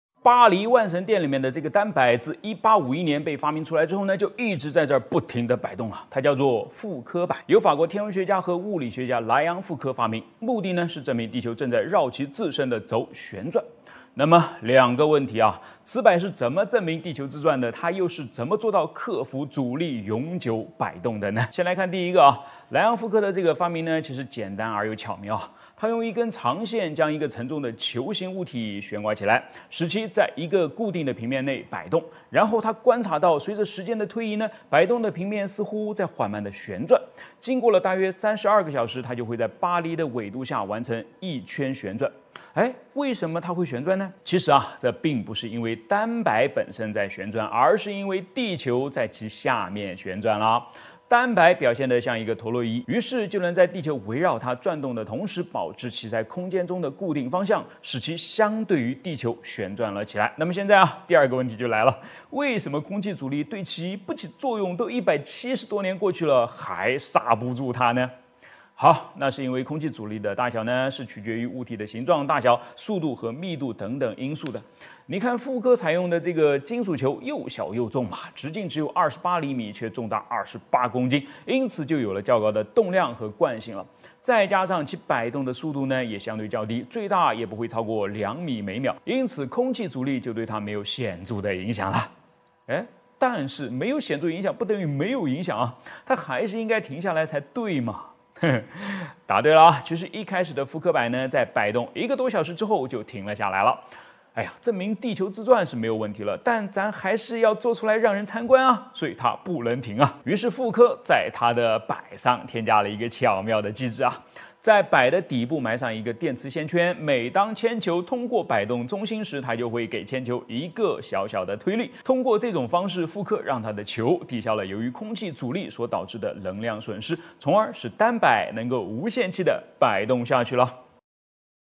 巴黎万神殿傅科摆停止过工作吗？真是“永动机”？它是如何证明地球自转的？ 【科学火箭叔】
本频道制作的所有视频都是为了向观众传递有趣的科普知识，绝对符合平台的使用政策。 大家好，我是你们的“百科全叔”火箭叔~